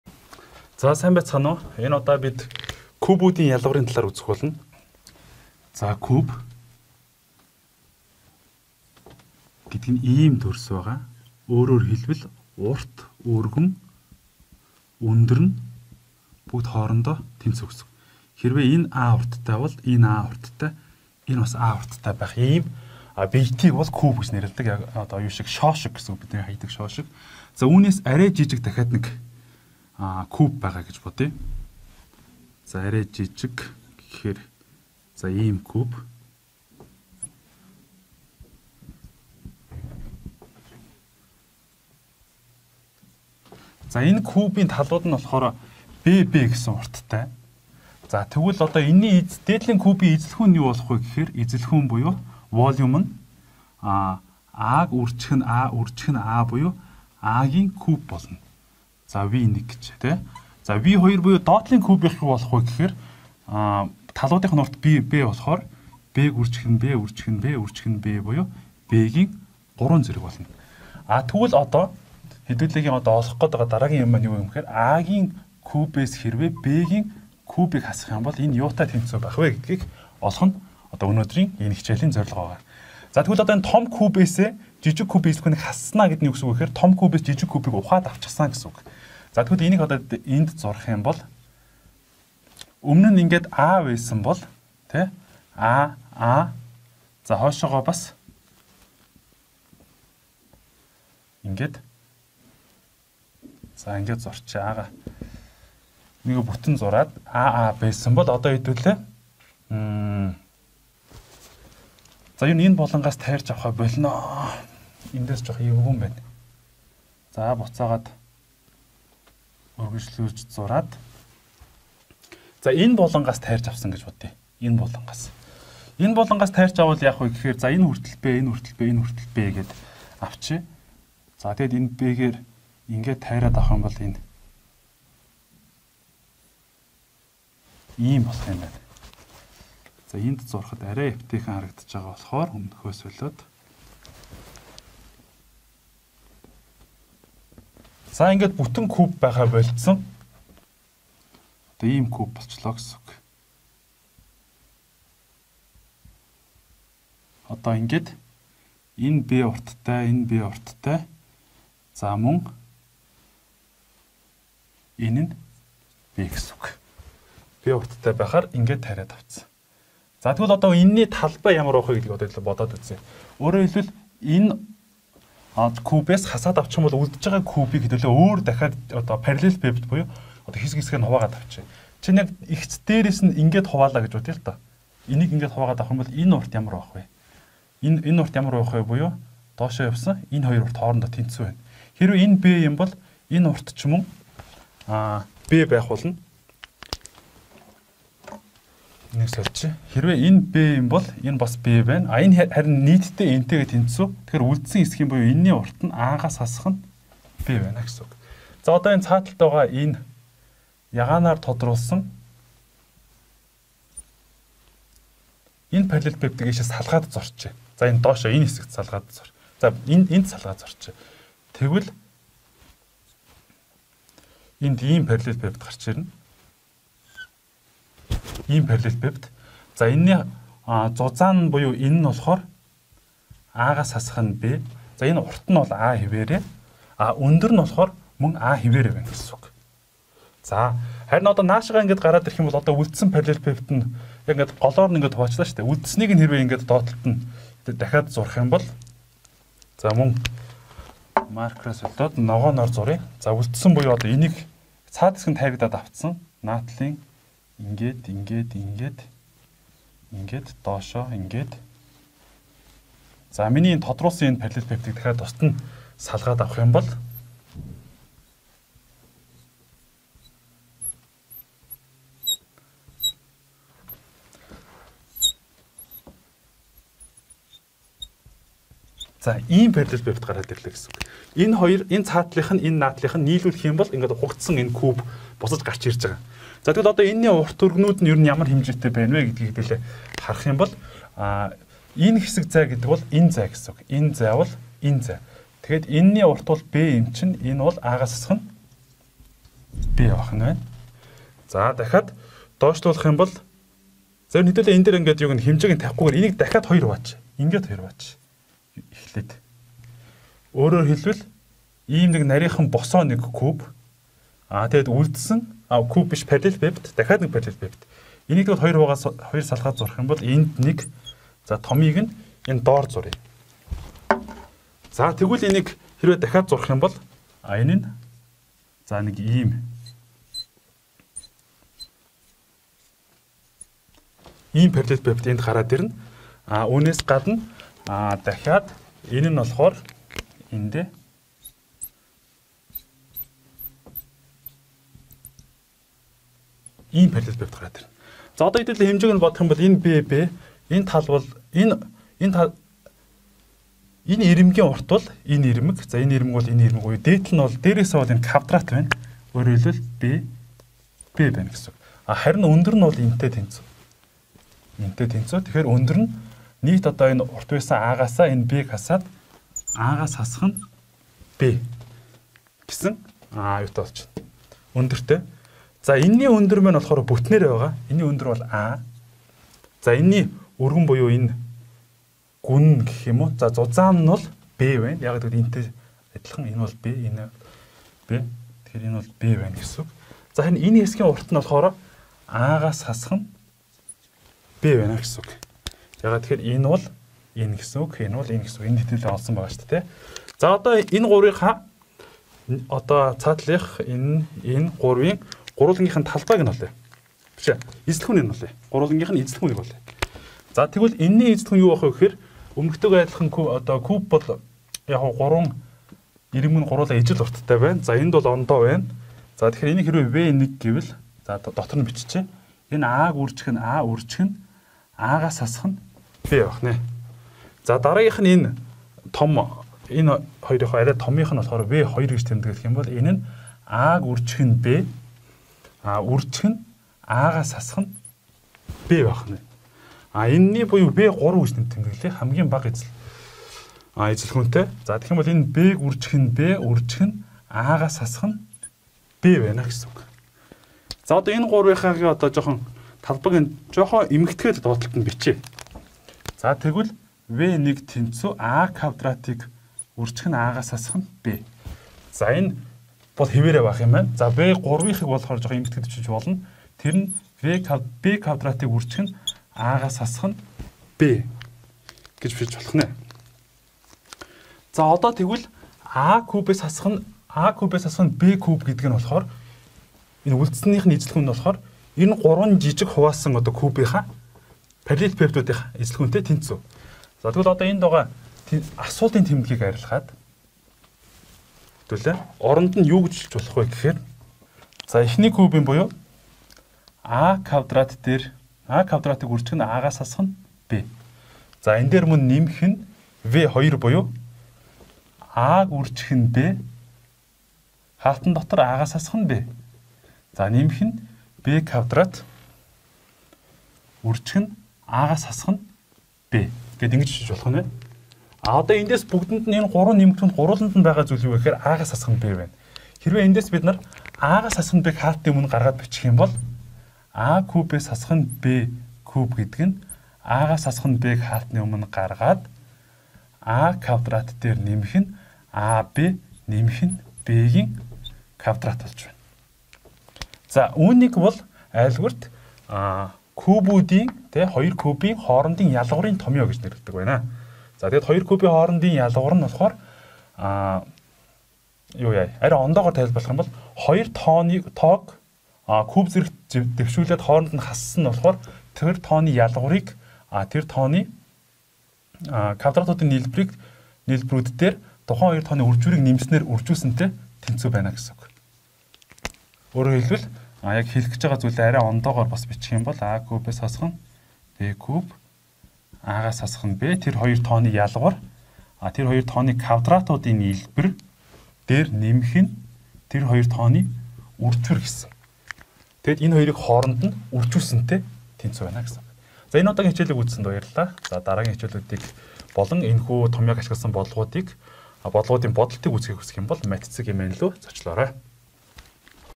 Кубуудын ялгаврын томьёо геометр утга - Бямбаа | Byambaa | Онлайн хичээл
Энэ удаагийн видео хичэлээр кубуудын томьёог геометр утгаар нь хэрхэн олох талаар оруулж байна.